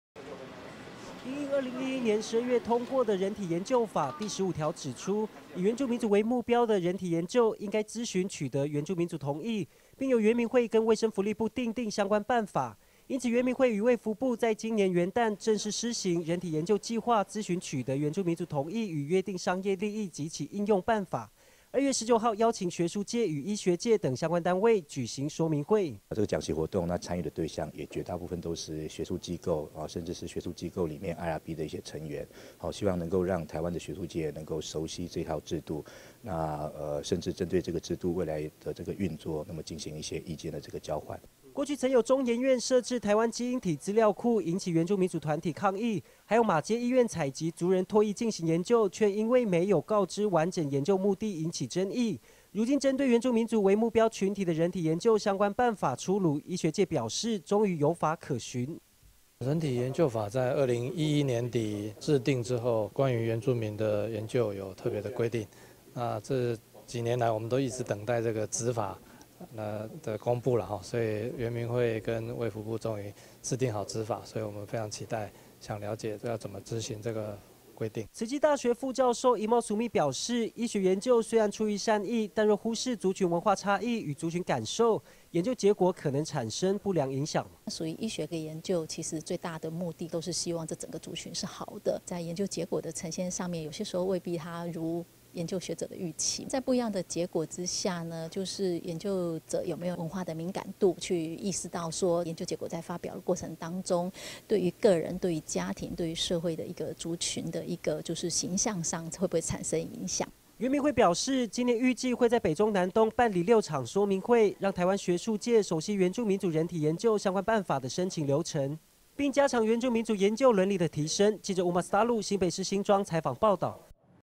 原民人體研究同意辦法 今舉行說明會 2016-02-19 TITV 原視新聞
因應2011年12月通過的人體研究法，-第15條指出，以原住民族為目標的人體研究，應該諮詢取得原住民族同意，並由原民會跟衛生福利部訂定相關辦法-。因此，原民會與衛福部在今年元旦，正式施行「人體研究計畫諮詢取得原住民族同意與約定商業利益及其應用辦法」，2月19號邀請學術界與醫學界等相關單位，舉行說明會。

過去曾有中研院設置「台灣基因體資料庫」引起原住民族團體抗議，還有馬偕醫院採集族人唾液進行研究，卻因為沒有完整告知研究目的引起爭議，如今針對原住民族為目標群體的人體研究相關辦法出爐，醫學界表示終於有法可循。

慈濟大學副教授怡懋‧蘇米表示，醫學研究雖然出於善意，但若忽視族群文化差異與族群感受，研究結果可能產生不良影響。

原民會表示，今年預計會在北中南東辦理六場說明會，讓台灣學術界熟悉原住民族人體研究相關辦法的申請流程，並加強原住民族研究倫理的提升。